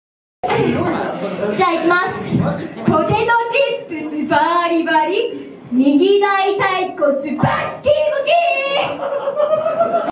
Happy少女♪愛沢さいりによる一発芸（2015/3/1Team くれれっ娘！ × HAPPY少女ツーマンライブ）
Happy少女♪リーダーさいりによる、渾身の一発芸！！